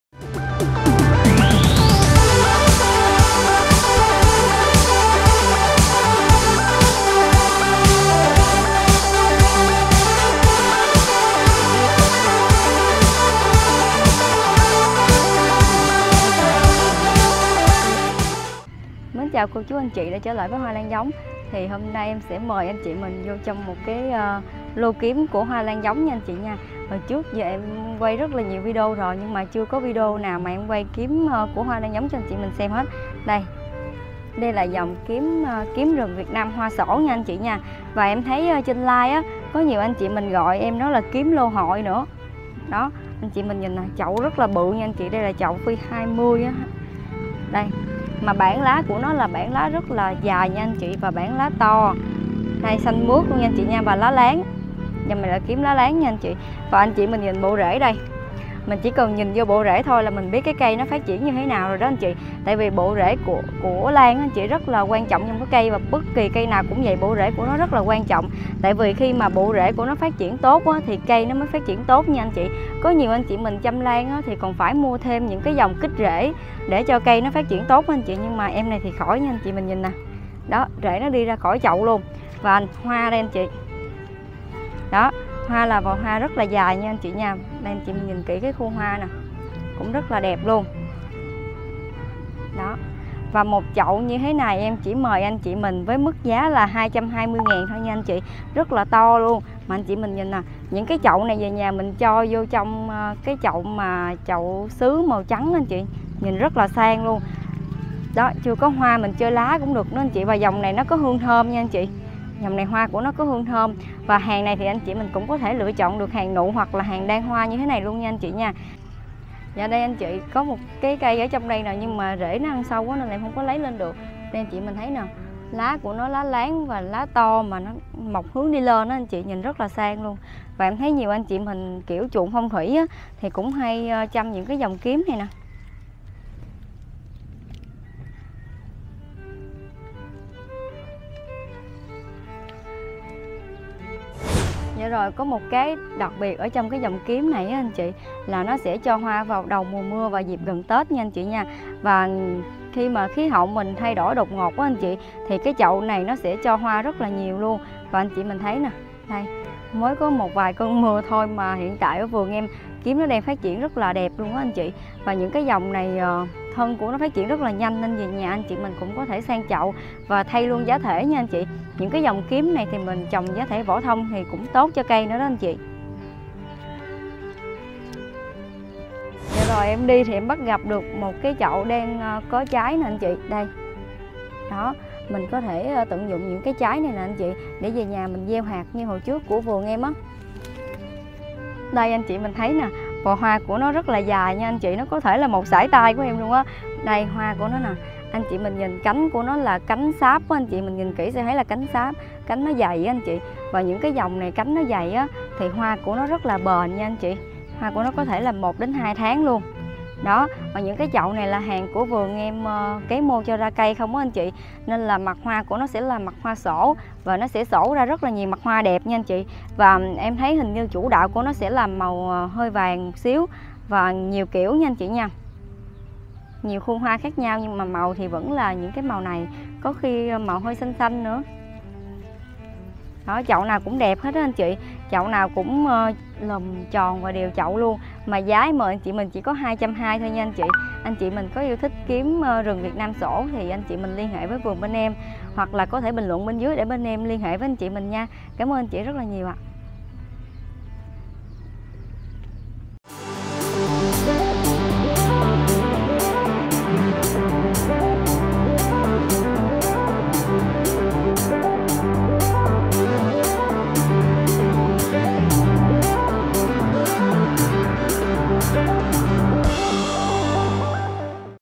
Hoa Lan Kiếm Rừng Xổ Việt Nam
Ngoài các dòng lan kiếm Phan Trí có màu sắc nổi bậc. Kiếm Xanh Huế, Kiếm Sơn, Kiếm Như Quỳnh còn có dòn kiếm rừng xổ có màu sắc vàng tươi tắn cùng với lưỡi đốm lạ mắc sẽ là một dòng lan đáng sưu tầm đó ạ.
⭐ Địa chỉ: 49 Bà râm, Tân điền, Long thượng, Cần giuộc, Long an